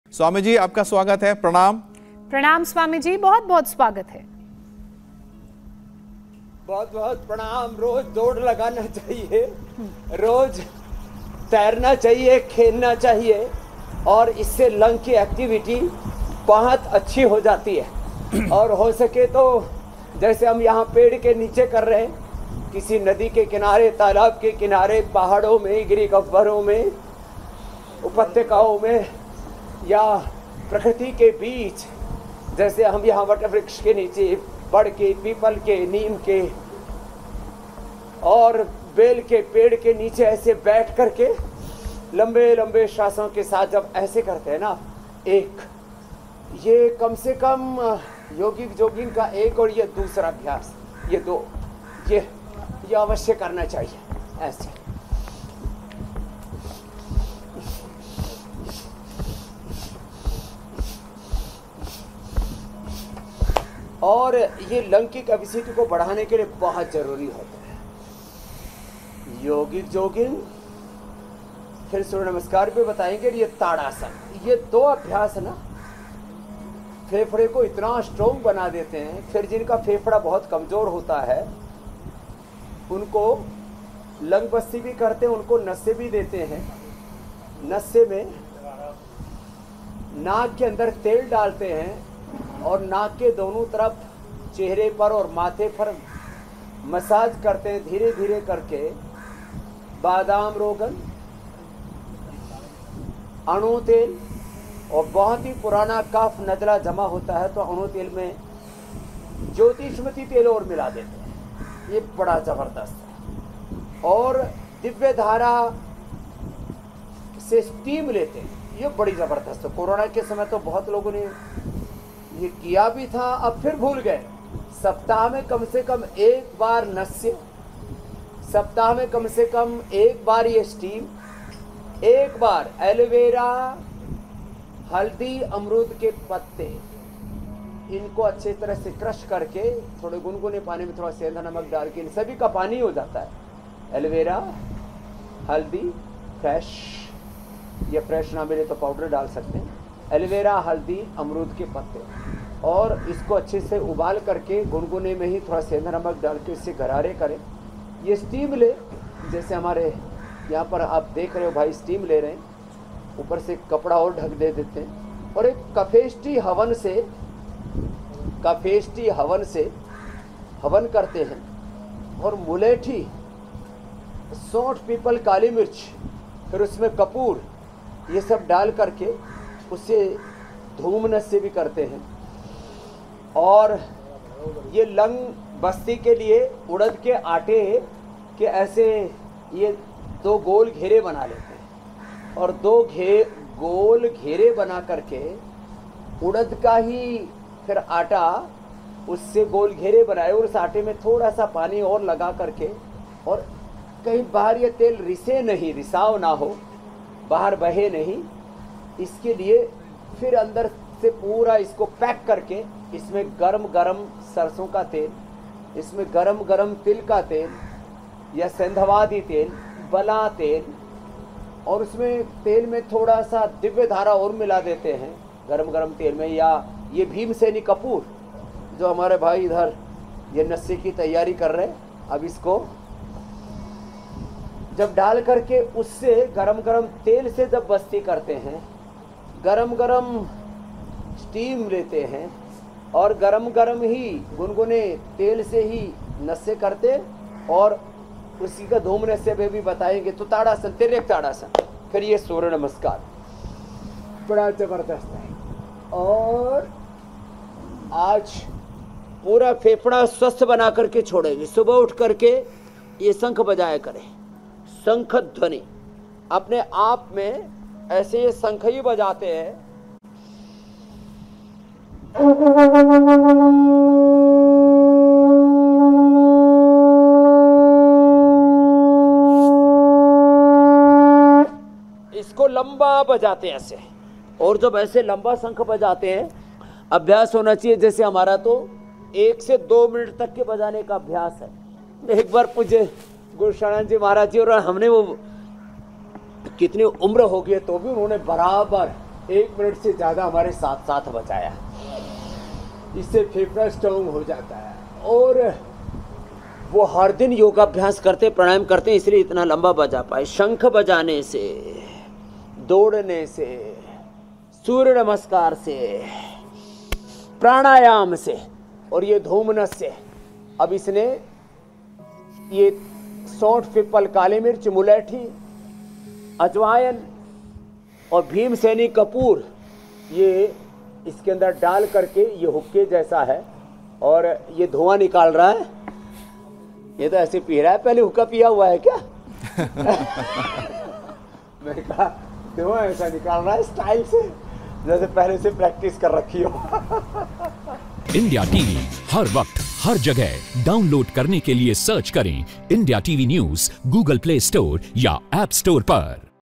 Yoga for Lungs: Lungs को कैसे बनाएं मजबूत? जानिए Swami Ramdev से योगिक Formula | IndiaTV Yoga
जहरीली हवा लोगों की उम्र खा रही है...और ये वाकई बड़ा खतरा है। क्योंकि मुश्किल से चार महीने पहले ही द नेचर मेडिकल जर्नल ने भी अपने कवर पेज पर दिल्ली के एयर पॉल्यूशन की तस्वीर पब्लिश की थी और जहरीली हवा से लंग्स कैंसर होने का दावा किया था।